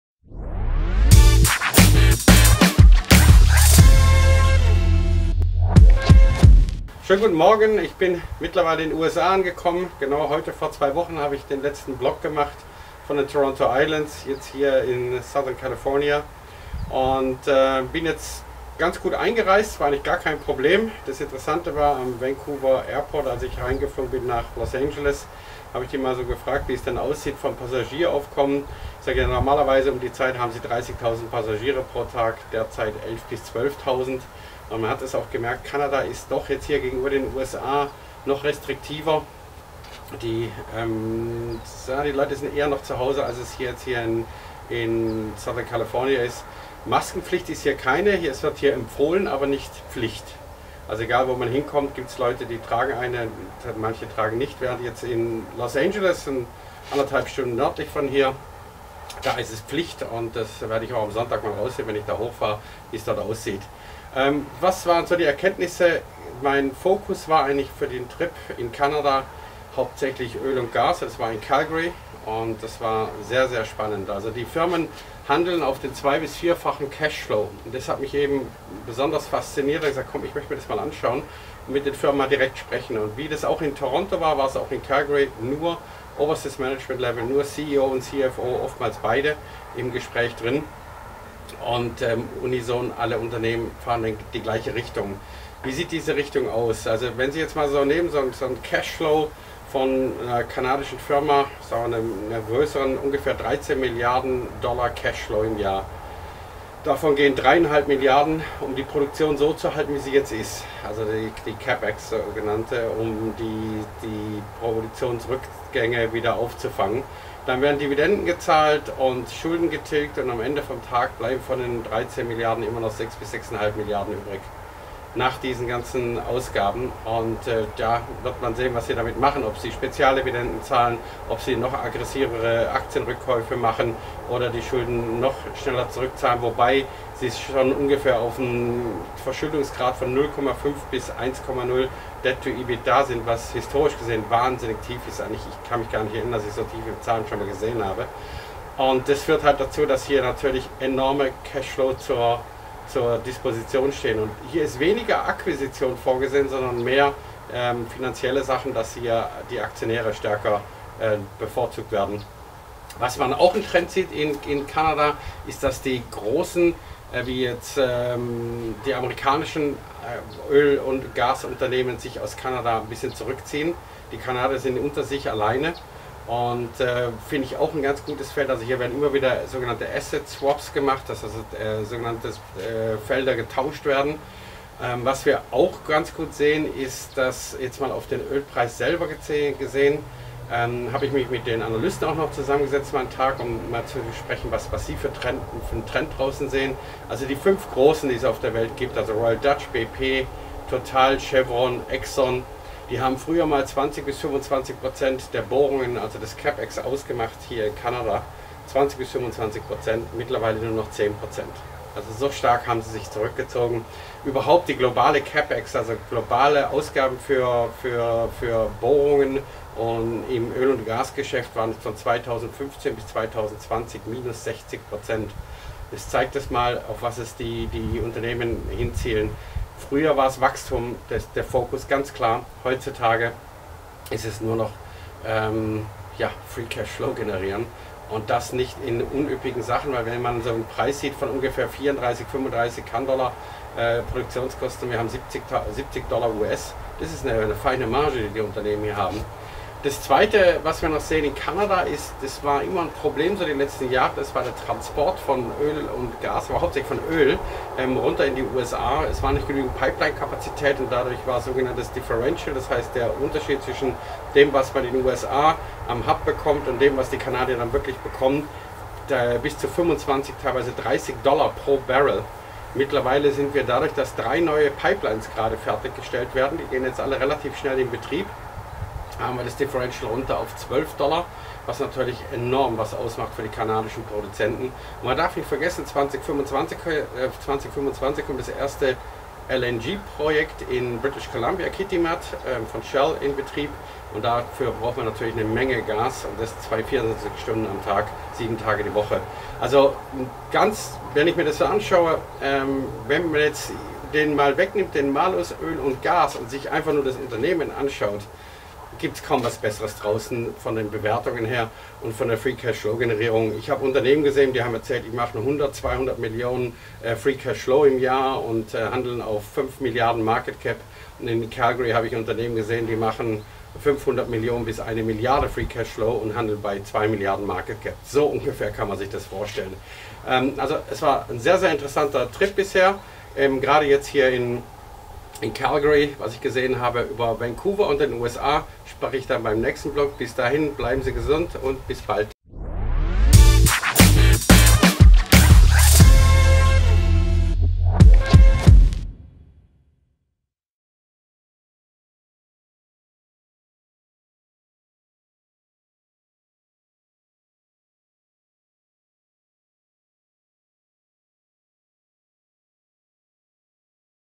TBF VLOG // Kalifornien // September 2021
und weiter geht´s auf dem fieldtrip durch Nordamerika // Peter Dreide, Inhaber und CIO von TBF, berichtet von seinen Eindrücken aus Kanada und Kalifornien